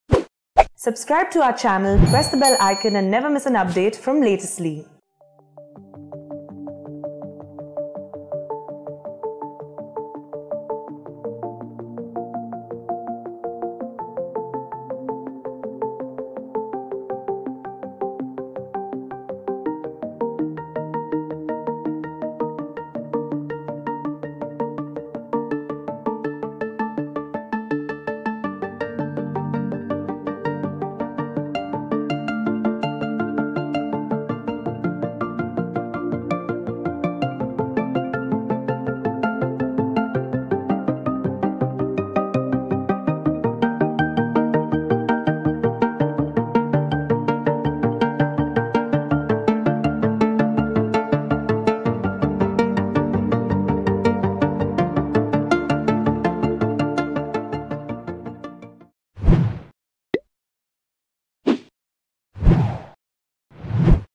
Disease X: कोरोनापेक्षा 7 पट जास्त धोकादायक महामारी येणार, शास्त्रज्ञांनी व्यक्त केली भीती
जग कोरोना विषाणूच्या साथीतून अजूनही पूर्णपणे सावरलेले नाही, अशात तज्ञांनी नवीन महामारी येण्याची भीती व्यक्त केली आहे. पुढील महामारीत किमान 5 कोटी लोकांचा मृत्यू होऊ शकतो, असा दावा केला जात आहे, जाणून घ्या अधिक माहिती